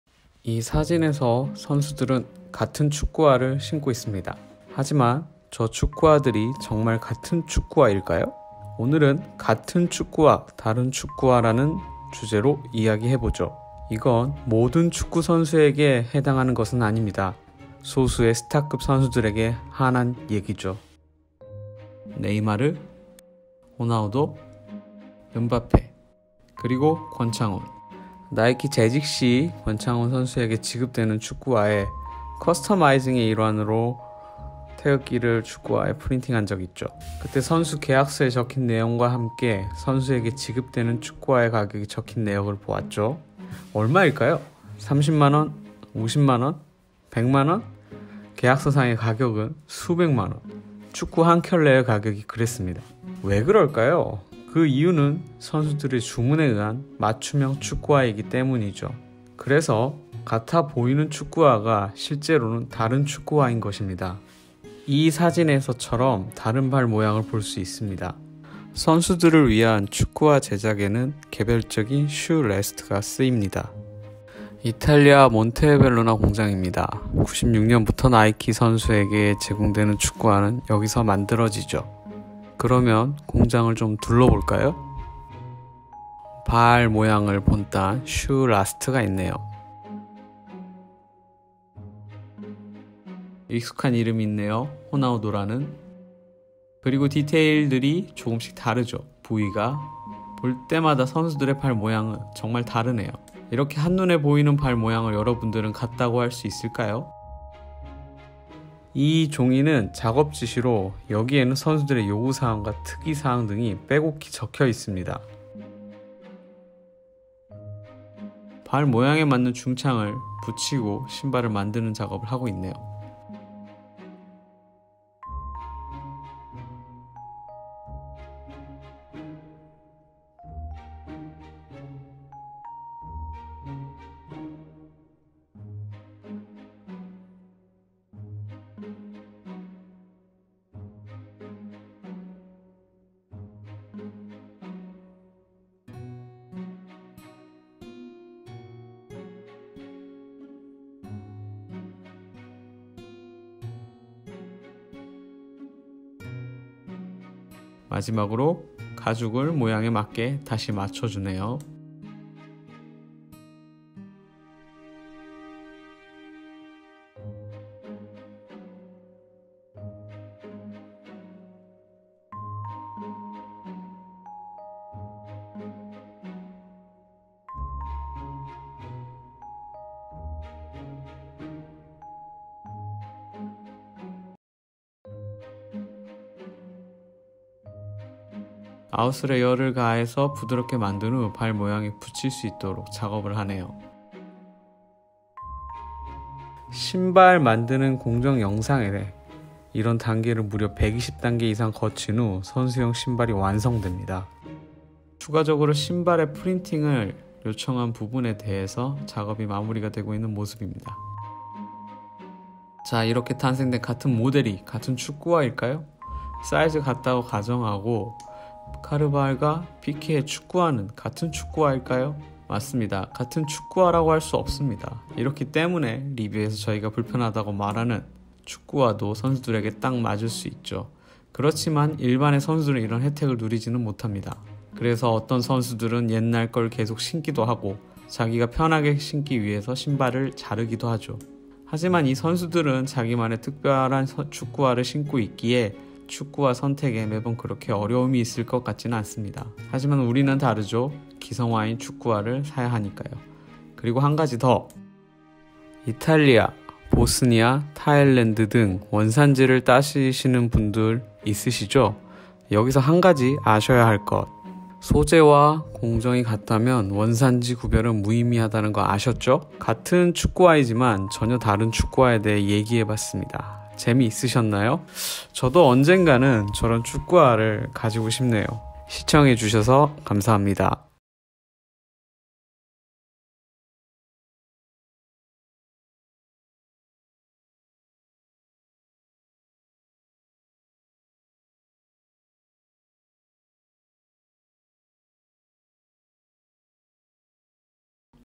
같은 축구화 다른 축구화- 나이키 선수지급용
수많은 스타급 선수들이 사용하는 축구화는 몇가지로 같은 축구화를 신고 있는 선수들이 많이 있습니다. 그런데 이들이 신고 있는 같은 종류의 축구화가 정말 같은 축구화일까요? 오늘은 나이키 계약선수들의 축구화 제조과정을 알아보겠습니다.